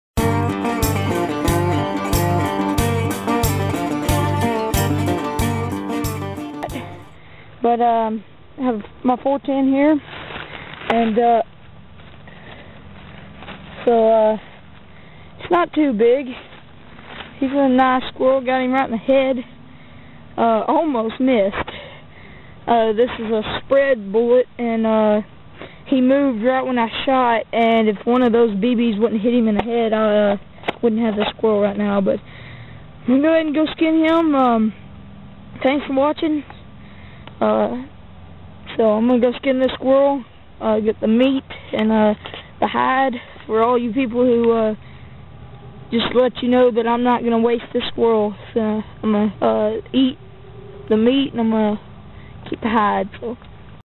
Squirrel Kill with the .410 Pump Action Shotgun
Tyler Shane killed a nice squirrel with his .410.